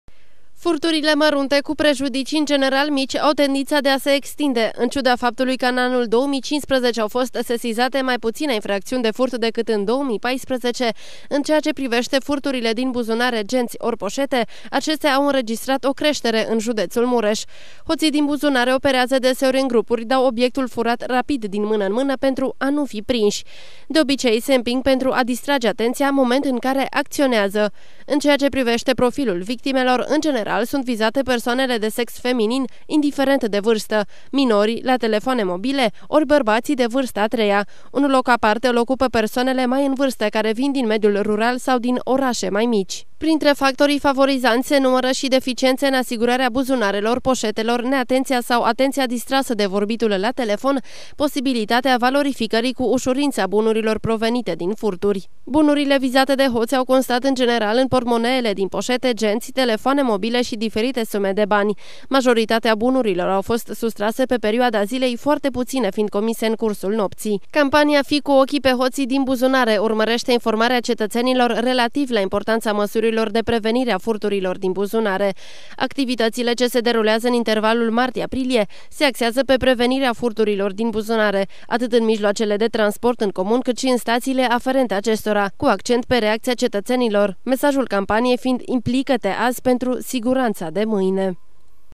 „FII CU OCHII PE HOŢII DIN BUZUNARE!” (2016 04 08)
Hoţii din buzunare sunt unii dintre cei mai activi în rândul populaţiei. Furturile de acest gen au înregistrat o creştere în anul 2015 în judeţul Mureş. Hoţii din buzunare operează deseori în grupuri, dau obiectul furat rapid din mână în mână pentru a nu fi prinşi! De obicei se împing pentru a distrage atenţia, moment în care acţionează! În ceea ce priveşte profilul victimelor, în general sunt vizate persoanele de sex feminin indiferent de vârstă, minorii şi bărbaţii de vârsta a treia. Campania „FII CU OCHII PE HOŢII DIN BUZUNARE!” urmăreşte informarea cetăţenilor relativ la importanţa măsurilor de prevenire a acestor evenimente.